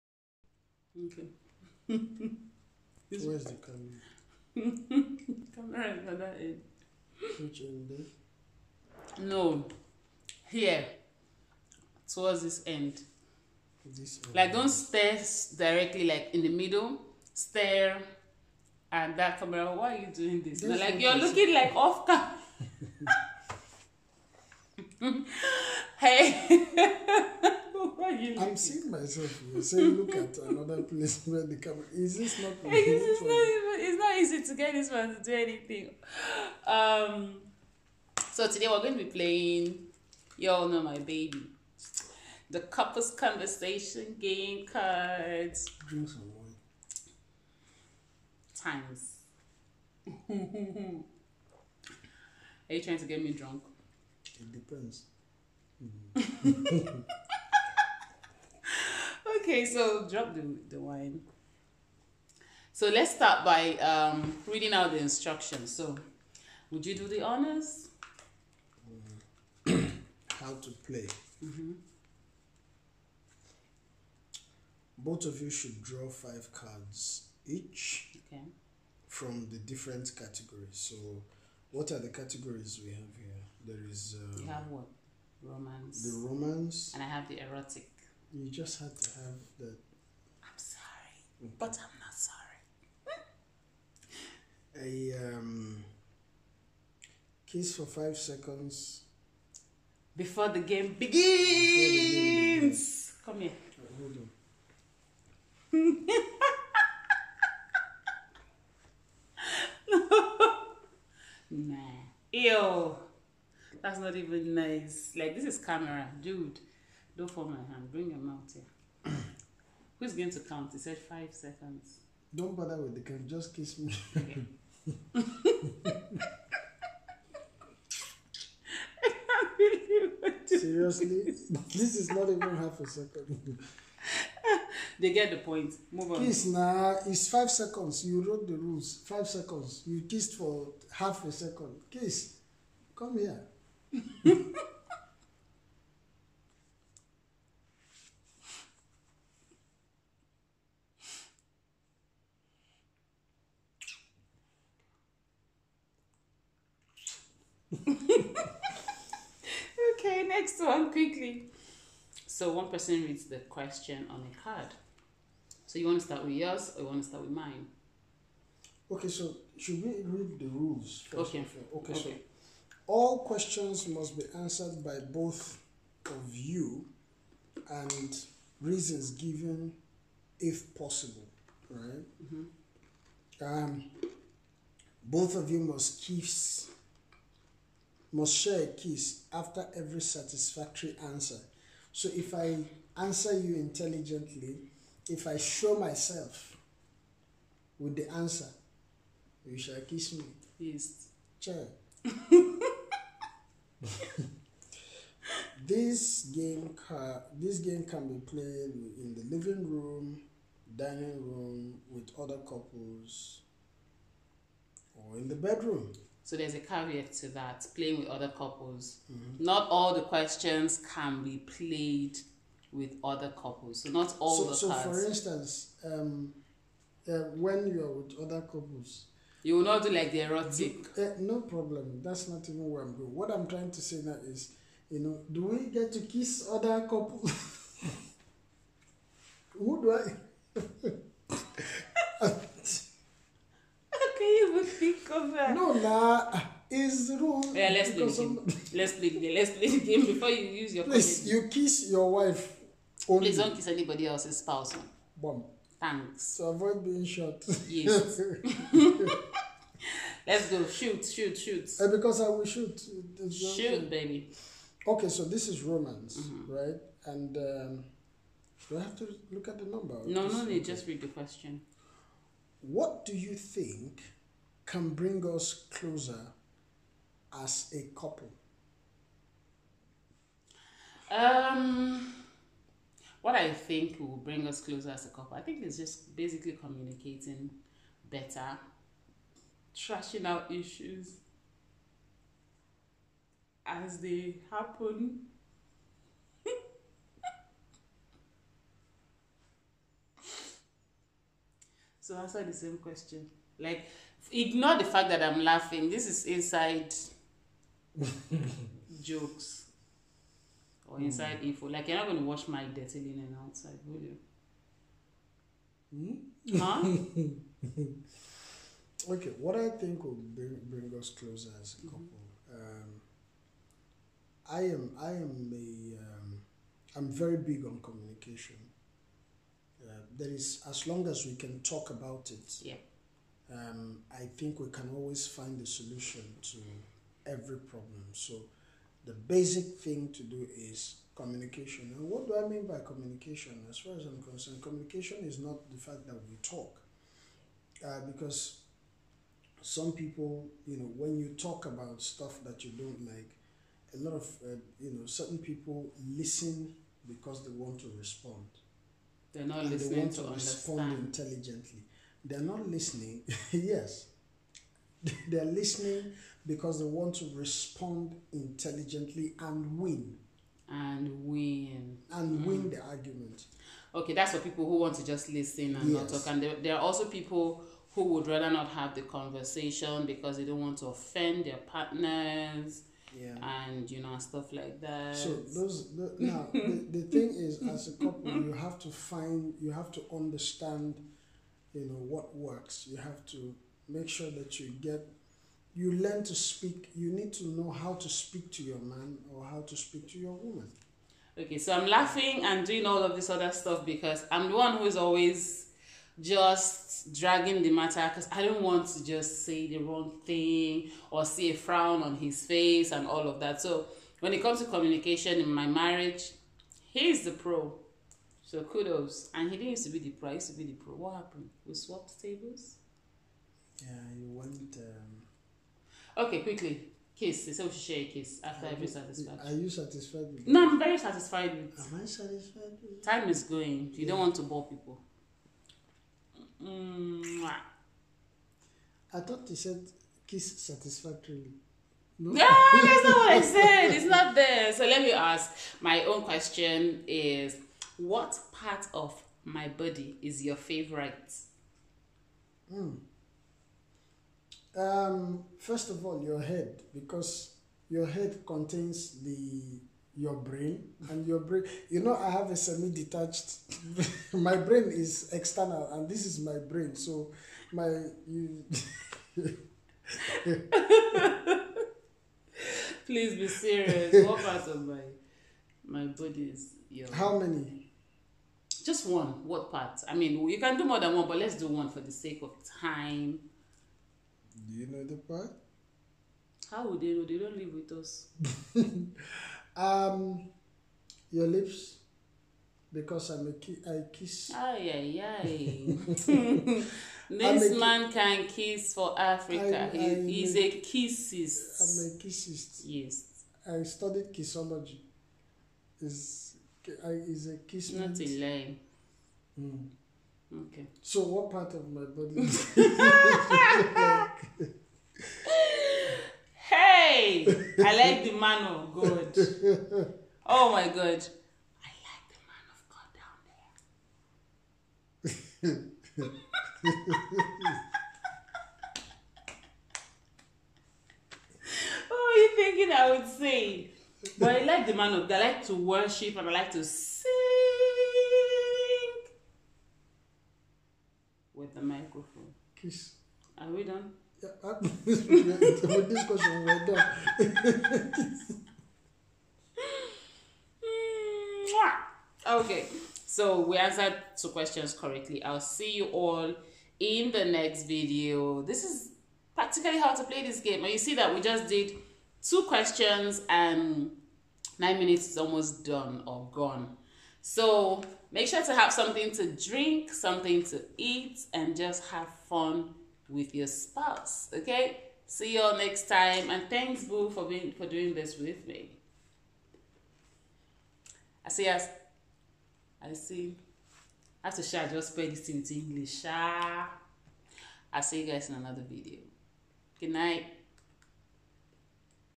Couple's Conversation Game Cards!
How to create fun moments with your spouse. Date night idea.